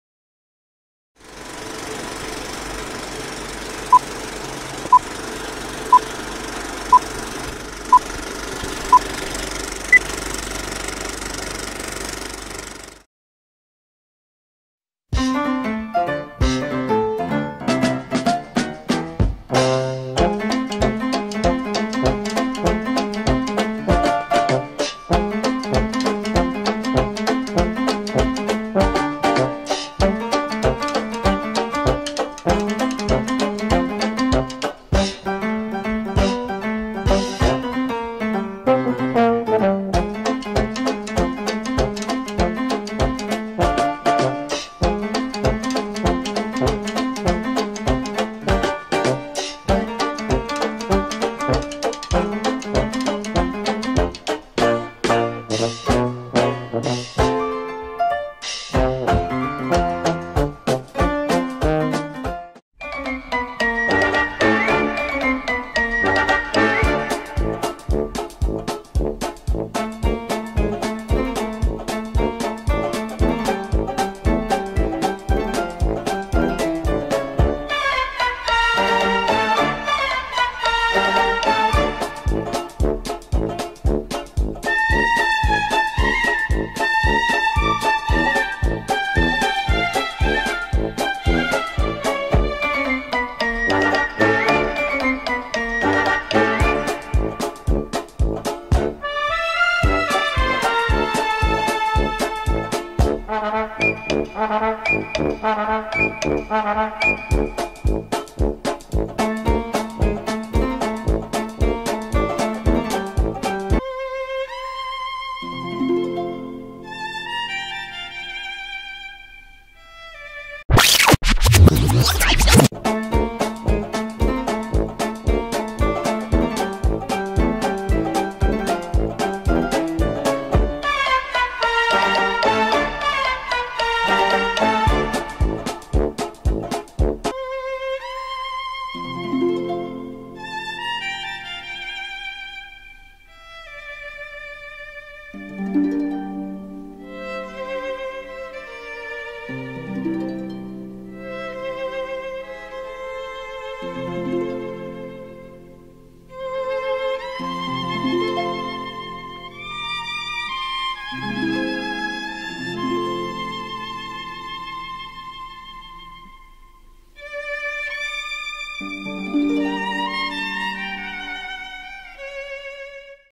The Dog, the Hat and the Umbrella
In this silent film, three girls that become unlikely allies after some of their most prized possessions are swept away and stolen in a blink of an eye.

Created in association with a Broken Tart Production.